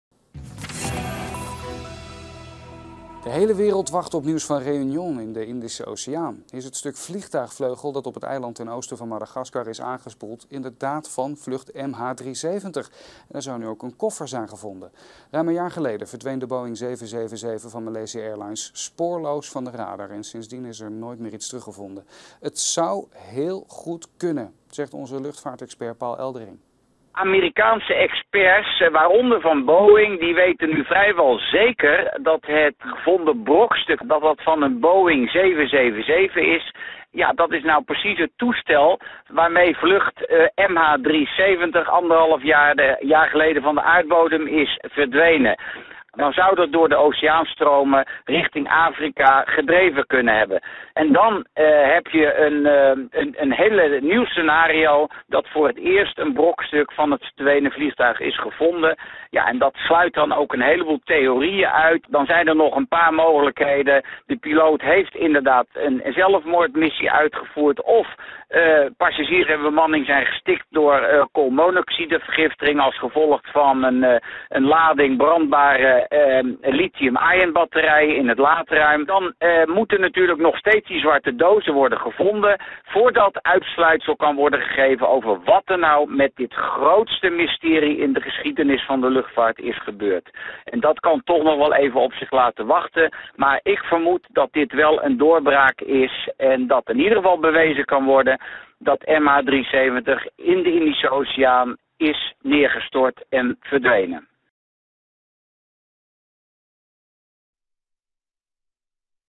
‘Vondst vleugel is doorbraak’
Is het stuk vleugel op het eiland Réunion inderdaad van de vermiste vlucht MH370? Sommige experts zijn al overtuigd. Luchtvaartredacteur Paul Eldering heeft het laatste nieuws.